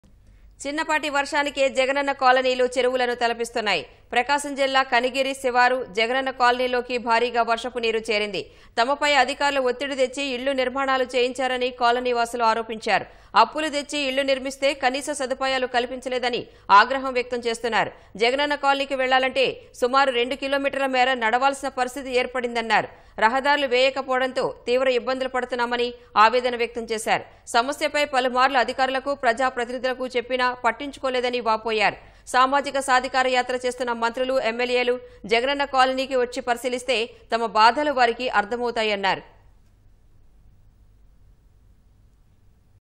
చెరువుల్లా జగనన్న కాలనీలు | Jagananna Colonies Filled up with Flood Water | Kanigiri
చిన్నపాటి వర్షానికే జగనన్న కాలనీలు చెరువులను కలపిస్తున్నాయి. ప్రకాశం జిల్లా కనిగిరి శివారు జగనన్న కాలనీలోకి భారీగా వర్షాపు నీరుచేరింది.
తమపై అధికారులు ఒత్తిడి తెచ్చి ఇళ్లు నిర్మాణాలు చేయించారని కాలనీవాసులు ఆరోపించారు. అప్పులు తెచ్చి ఇళ్లు నిర్మిస్తే కనీస సదుపాయాలు కల్పించలేదని ఆగ్రహం వ్యక్తంచేస్తున్నారు. జగనన్న కాలనీకి వెళ్లాలంటే సుమారు రెండు కిలోమీటర్ల మేర నడవాల్సిన పరిస్థితి ఏర్పడిందన్నారు. రహదారులు వేయకపోవడంతో  తీవ్రఇబ్బందులు పడుతున్నామని ఆవేదన వ్యక్తంచేశారు